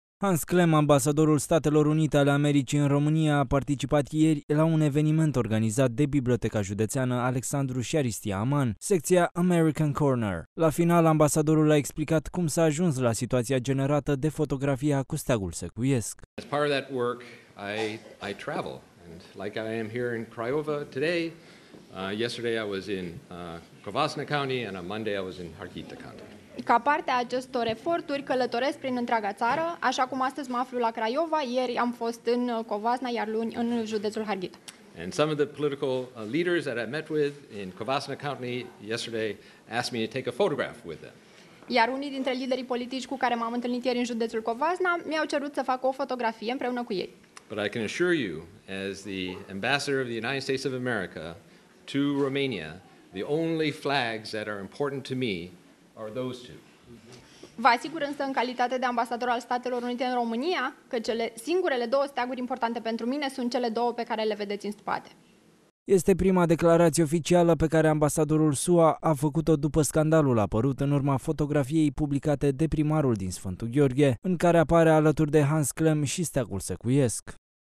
Unde duce politeţea...
"Fotografia cu steagul secuiesc a fost doar un gest de politete". A fost prima declaratie oficială a ambasadorului Americii în România,  Hans Klemm, după scandalul provocat de fotografia facută cu primarul din Sf Gheorghe in care apare cu steagul regiunii. Declaratia a fost facuta aseara la Craiova.